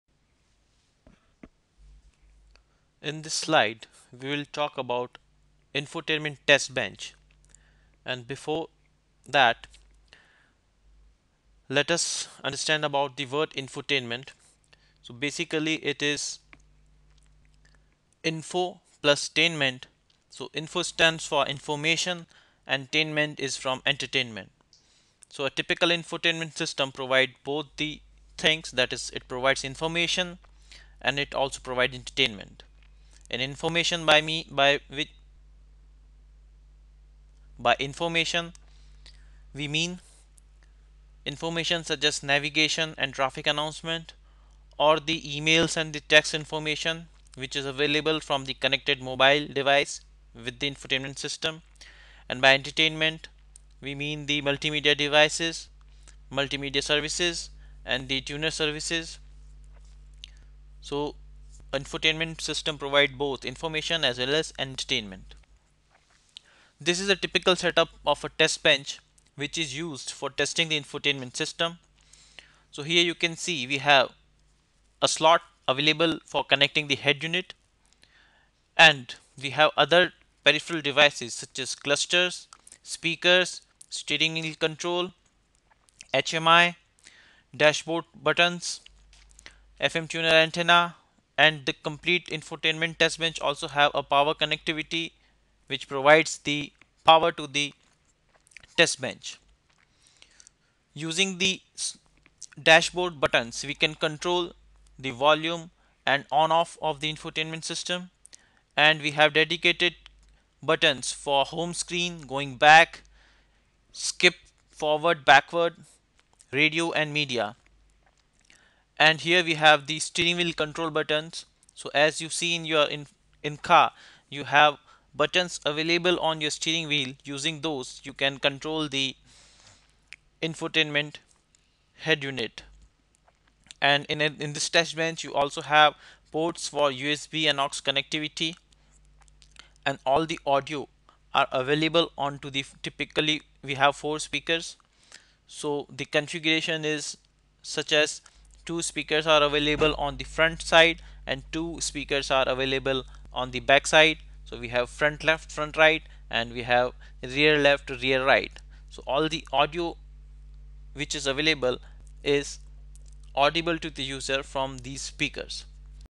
Automotive In Vehicle Infotainment IVI Test Bench
Automotive Infotainment also called as In Vehicle Infotainment  or IVI
In this video we discuss about the Infotainment Testing and Infotainment Test Bench Setup

Automotive Electronics Training Video
Automotive Infotainment Training Video